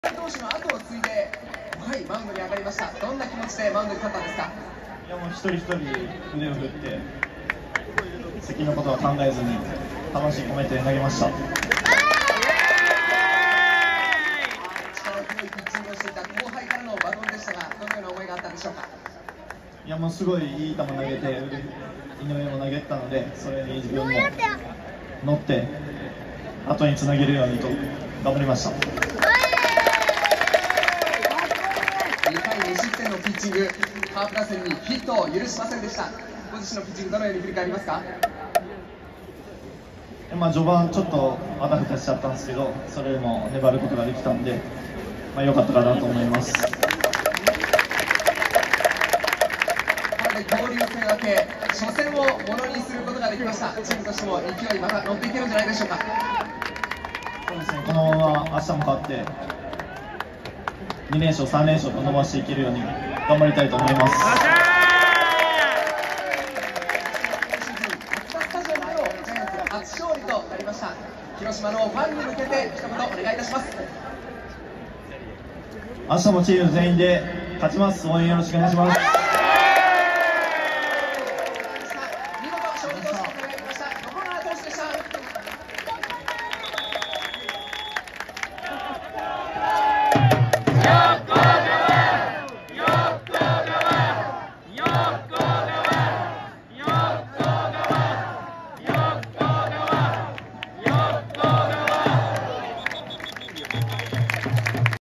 20230623 横川凱 ヒーローインタビュー
ついにマツダスタジアムでもビジターへの￼インタビュー解禁！！！

4勝目おめでとうございます！！！